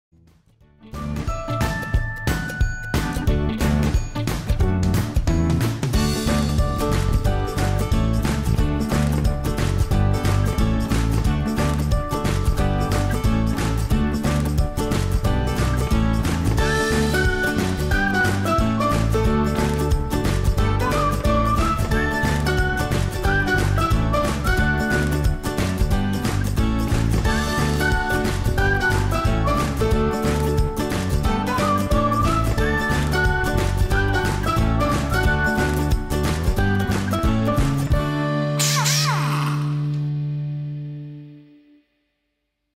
Werktafel met kruispoot 160 cm | Rintje Ritsma laat 't zien | Datona.nl | werkbank werkplaats
Rintje Ritsma laat deze werktafel met kruispoot 160 cm zien.

Ben je op zoek naar een werktafel dan heeft Datona deze natuurlijk voor jou.